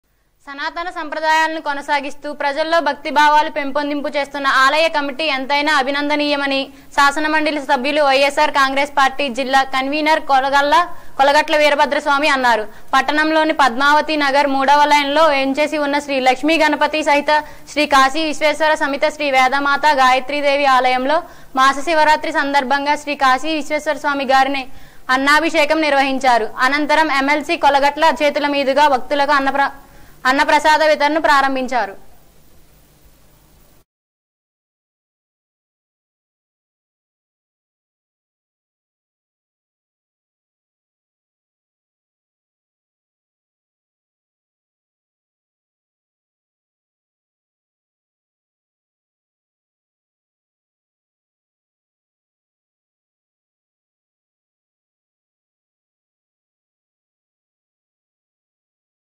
. సనాతన సంప్రదాయాలను కొనసాగిస్తూ ప్రజల్లో భక్తి భావాలు పెంపుదింపు చేస్తున్న ఆలయ కమిటీ
సనాతన సంప్రదాయాలను కొనసాగిస్తూ ప్రజల్లో భక్తి భావాలు పెంపుదింపు చేస్తున్న ఆలయ కమిటీ ఎంతైనా అభినందనీయమని శాసనమండలి సభ్యులు వైఎస్ఆర్ కాంగ్రెస్ పార్టీ జిల్లా కన్వీనర్ కోలగట్ల వీరభద్రస్వామి అన్నారు.  పట్టణంలోని పద్మావతి నగర్ 3వ లైన్ లో వేంచేసి ఉన్న శ్రీ లక్ష్మీ గణపతి సహిత శ్రీ కాశీవిశ్వేశ్వర సమేత శ్రీ వేదమాత గాయత్రీ దేవి ఆలయంలో మాస శివరాత్రి సందర్భంగా శ్రీ కాశీవిశ్వేశ్వర స్వామి గారికి అన్నాభిషేకం నిర్వహించారు.అనంతరం ఎమ్మెల్సీ కోలగట్ల చేతులమీదుగా భక్తులకు అన్నప్రసాద వితరణ ను ప్రారంభించారు.